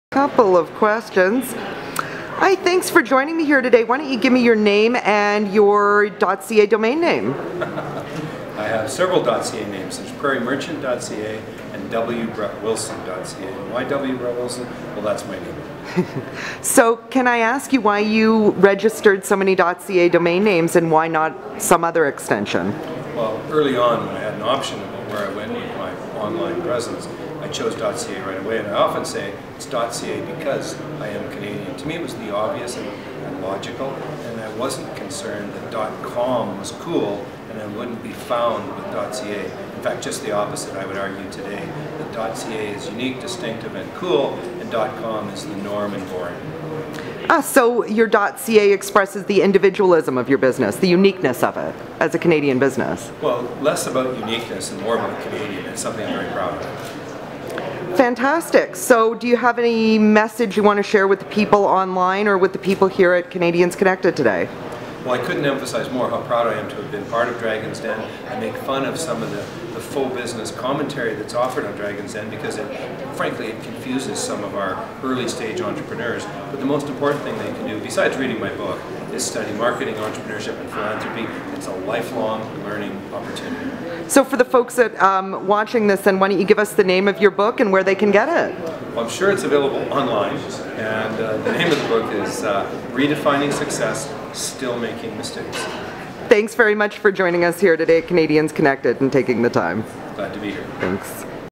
Brett Wilson for CIRA
Brett Wilson discusses how his .CA domain (wbrettwilson.ca) helps his business succeed.